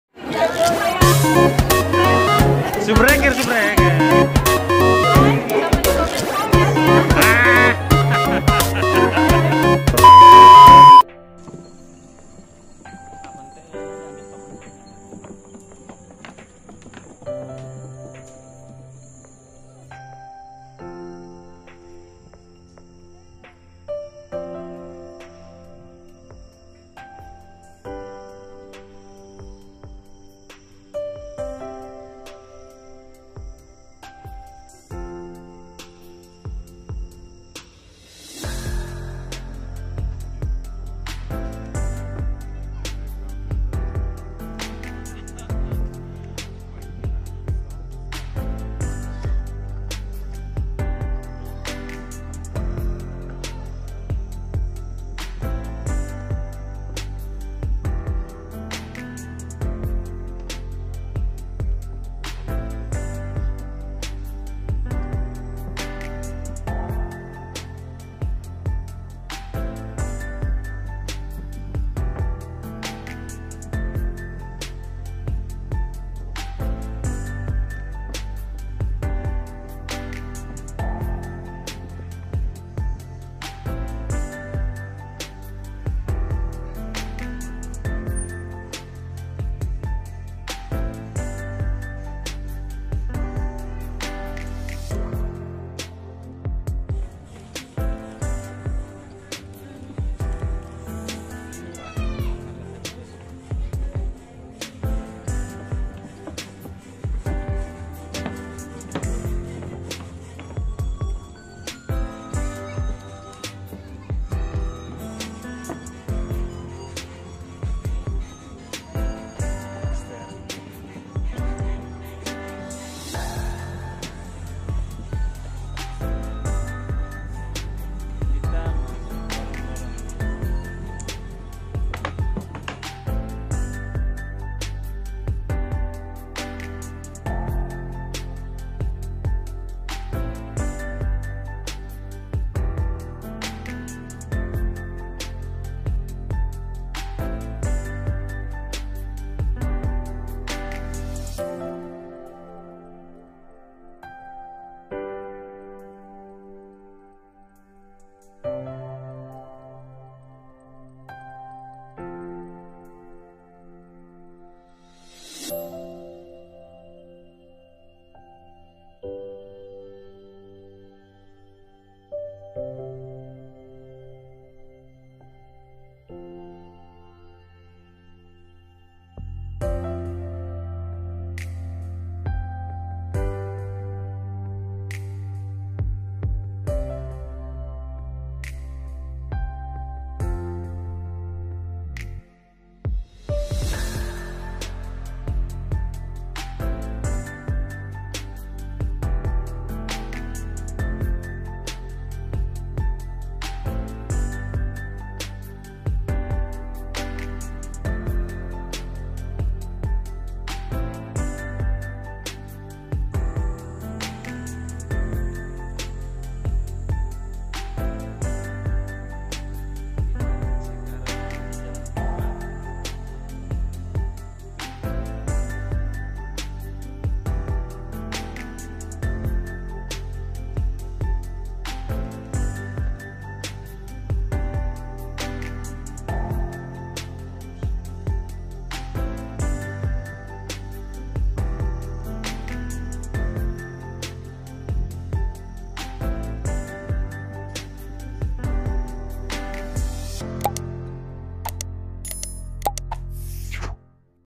Rejeki Tukang Ojek dapat Penumpang Cantik - Keliling Kota AGATS ASMAT PAPUA
Indahnya KOTA PAPAN, Keliling Kota AGATS ASMAT PAPUA.